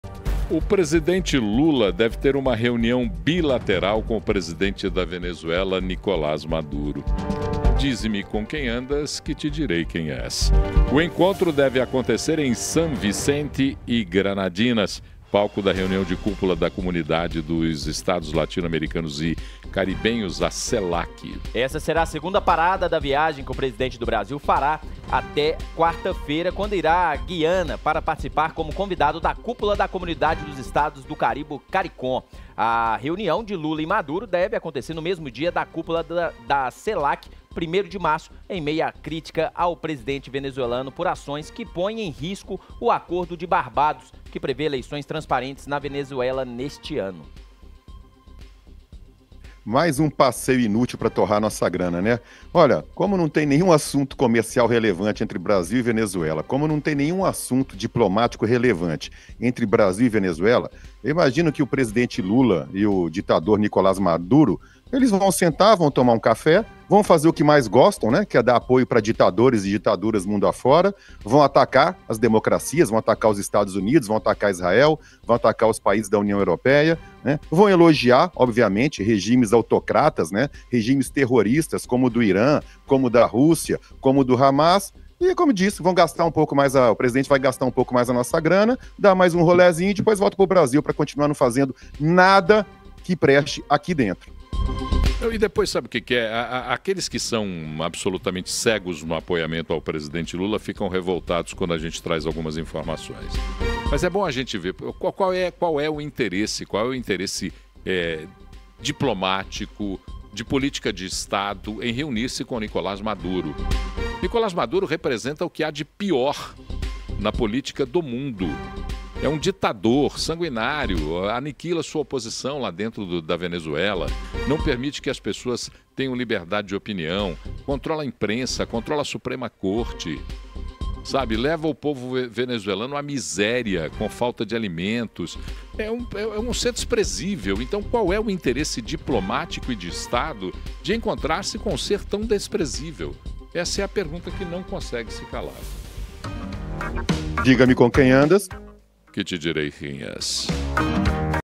LULA se encontrará com MADURO em meio a CRISE da VENEZUELA com a GUIANA
O presidente Lula se encontrará nesta semana com Nicolás Maduro para conversar sobre a crise diplomática entre Venezuela e Guiana. O encontro será realizado em São Vicente e Granadinas, onde acontecerá a cúpula da Comunidade dos Estados do Caribe, a Caricom, na qual Lula foi convidado. O presidente do Brasil também se reunirá com  Irfaan Ali, presidente da Guiana, para ouvir o outro lado do conflito.